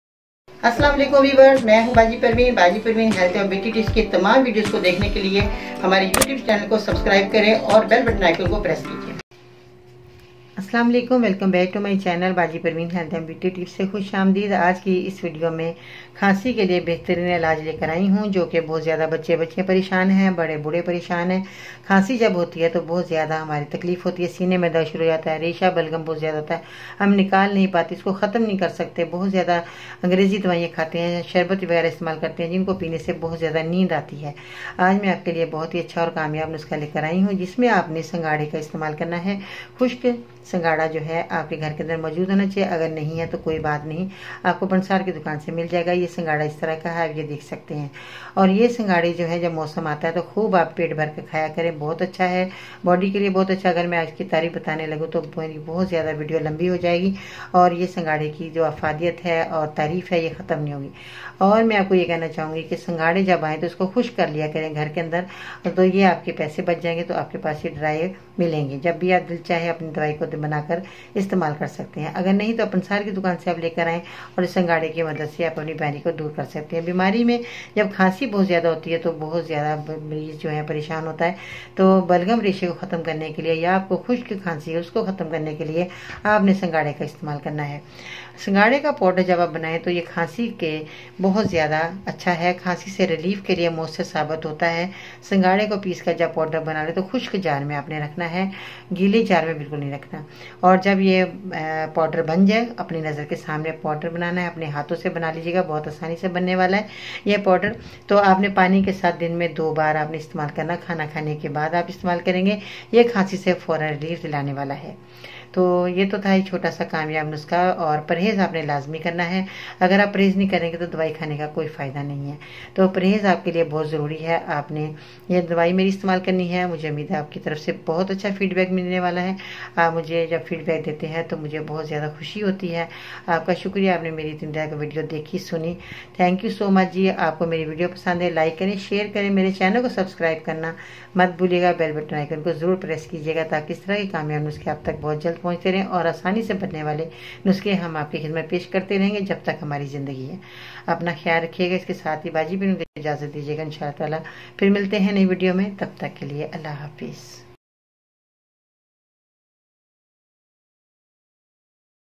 Home Remedies to Get Rid of a Cough - Kuta Khansi Ka Fori Ilaj
Pansarihome.com

 How to Cure Cough?
A cough is a reflex reaction designed to keep your airways clear. You may be coughing because of another condition, like asthma or a respiratory infection, or because you have swallowing difficulties
Ingredients:
 Dry Singhara
Make Fine Powder Of Dry Singhara. Use this Powder 2 Times in a Day after eating Food.